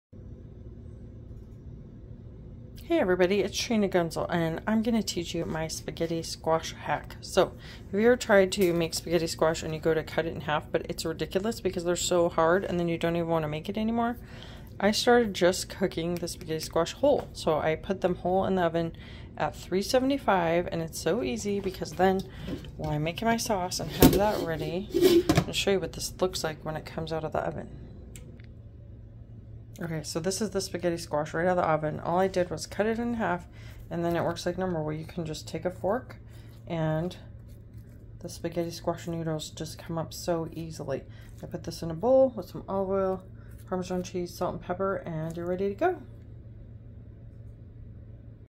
How to make spaghetti squash - simple hack and recipe FamilyFUNdamentals
Have you ever wanted to make spaghetti squash but struggled to cut it up or get it to taste well?

This recipe is so easy!

Bake at 375 for 2 hours on a cookie sheet, whole!

After, take out of the oven and let cool for 10 minutes.

Move to cutting board.

Now, you can easily cut it in half.

Scoop out the seeds.

Then, use a fork to scoop and shred the spaghetti squash; it literally pulls up like spaghetti.

Add to bowl.

Drizzle with olive oil and butter.

Sprinkle with salt and pepper.

Add fresh parmesan cheese, basil and slices cherry tomatoes.

Mix.

Serve with sauce is desired.

Join our Heartbeat 💓 community below for more recipes and family fun topics of interest.